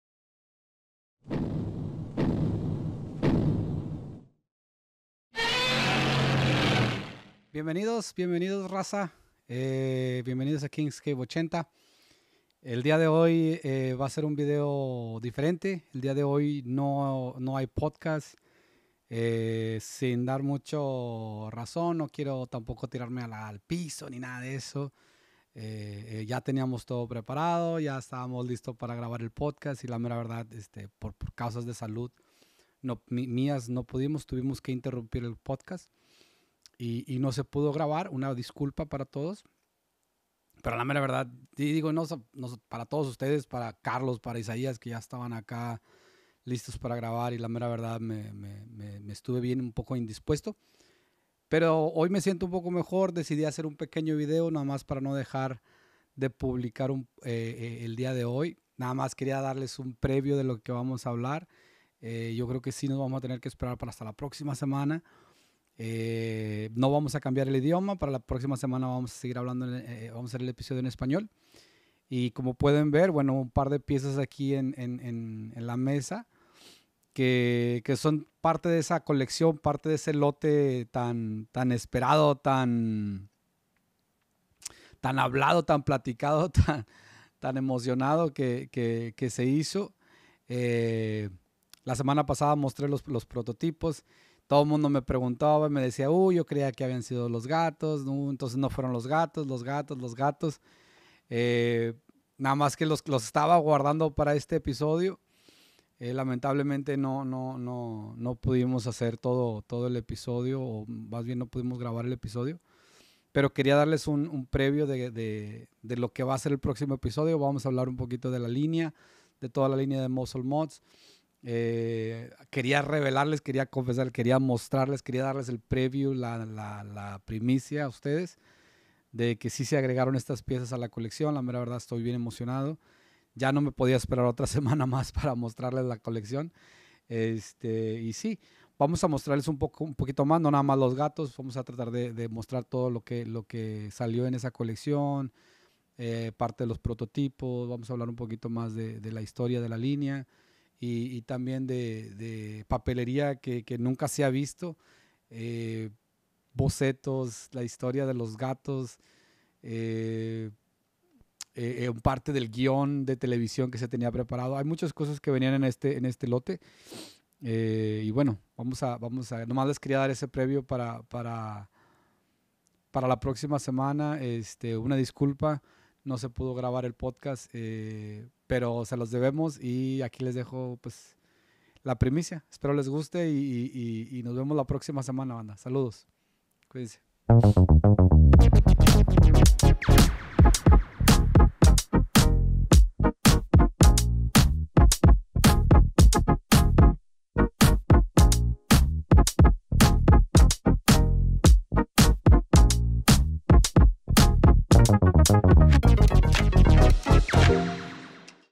Previo del Episodio 48
Por causas de salud, esta semana no pudimos grabar el episodio correspondiente, pero acá les dejo un previo de lo que vamos a hablar en el próximo capítulo. Espero les guste y nos vemos la próxima semana.